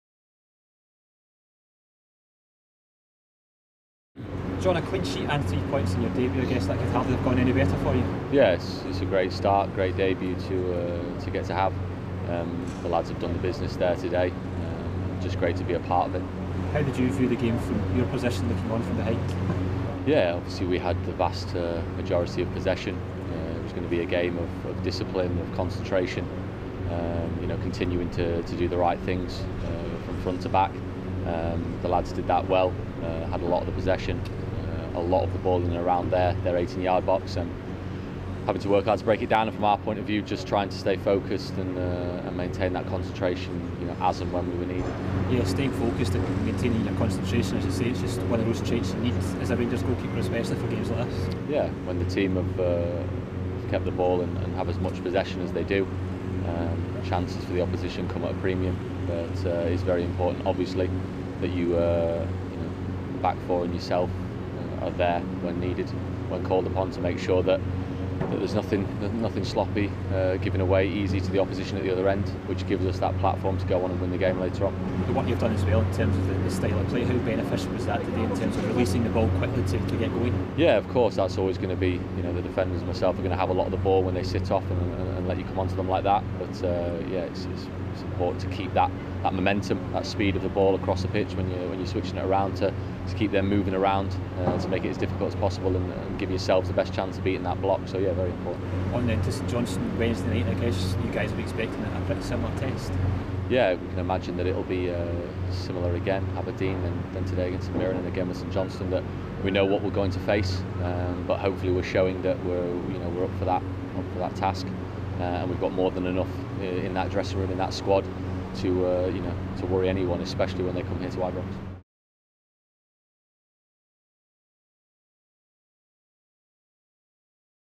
REACTION | Jon McLaughlin | Rangers 3-0 St Mirren
The keeper spoke to RTV after his competitive debut for the club